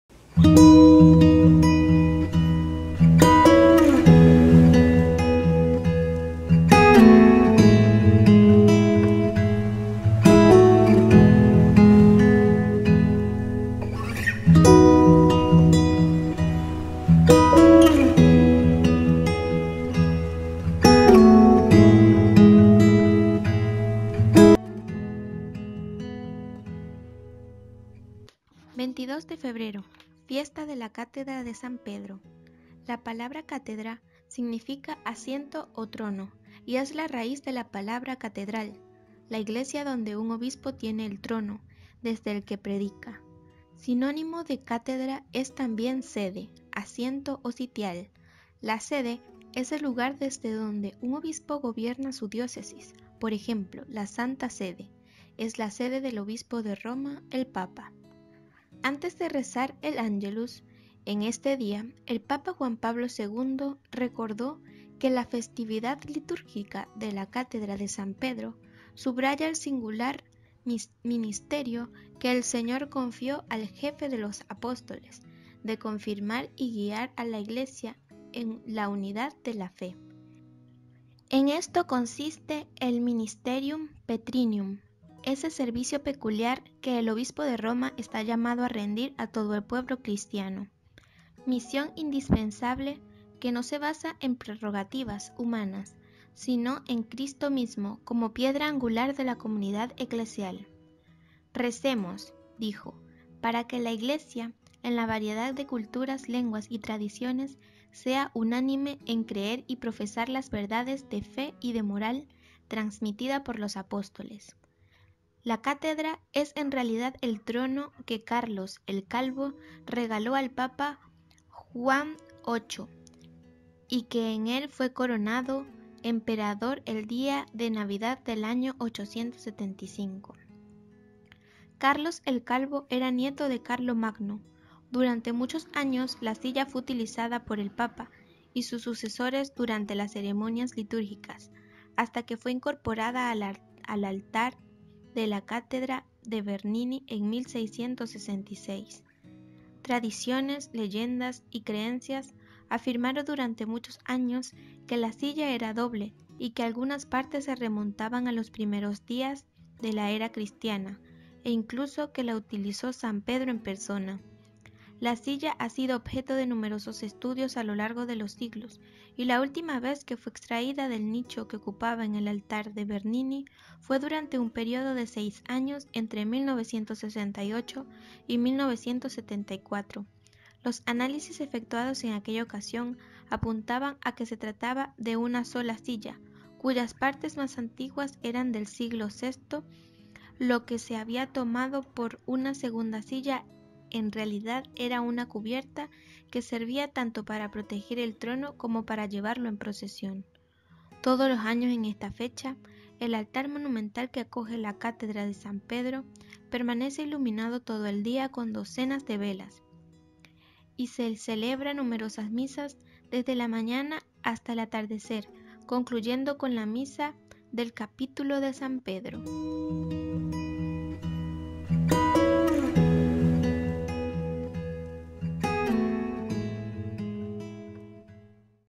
Fiesta de Hoy: 22 de Febrero Festividad de la Cátedra de San Pedro.
La palabra "cátedra" significa asiento o trono y es la raíz de la palabra catedral, la iglesia donde un obispo tiene el trono desde el que predica. Sinónimo de cátedra es también "sede" (asiento o sitial): la "sede" es el lugar desde donde un obispo gobierna su diócesis. Por ejemplo, la Santa Sede es la sede del obispo de Roma, el Papa.

Antes de rezar el Ángelus en este día, el Papa Juan Pablo II recordó que "la festividad litúrgica de la Cátedra de San Pedro subraya el singular ministerio que el Señor confió al jefe de los apóstoles, de confirmar y guiar a la Iglesia en la unidad de la fe. En esto consiste el 'ministerium petrinum', ese servicio peculiar que el obispo de Roma está llamado a rendir a todo el pueblo cristiano. Misión indispensable, que no se basa en prerrogativas humanas, sino en Cristo mismo como piedra angular de la comunidad eclesial". "Recemos -dijo- para que la Iglesia, en la variedad de culturas, lenguas y tradiciones, sea unánime en creer y profesar las verdades de fe y de moral transmitidas por los apóstoles".

La cátedra es en realidad el trono que Carlos el Calvo regaló al papa Juan VIII y en el que fue coronado emperador el día de Navidad del año 875. Carlos el Calvo era nieto de Carlomagno. Durante muchos años la silla fue utilizada por el papa y sus sucesores durante las ceremonias litúrgicas, hasta que fue incorporada al Altar de la Cátedra de Bernini en 1666.

Tradiciones, leyendas y creencias afirmaron durante muchos años que la silla era doble y que algunas partes se remontaban a los primeros días de la era cristiana e incluso que la utilizó San Pedro en persona. La silla ha sido objeto de numerosos estudios a lo largo de los siglos y la última vez que fue extraída del nicho que ocupa en el altar de Bernini fue durante un período de seis años, entre 1968 y 1974. Los análisis efectuados en aquella ocasión apuntaban a que se trataba de una sola silla cuyas partes más antiguas eran del siglo VI. Lo que se había tomado por una segunda silla era en realidad una cubierta que servía tanto para proteger el trono como para llevarlo en procesión.

Todos los años en esta fecha, el altar monumental que acoge la Cátedra de San Pedro permanece iluminado todo el día con docenas de velas y se celebran numerosas misas desde la mañana hasta el atardecer, concluyendo con la misa del Capítulo de San Pedro.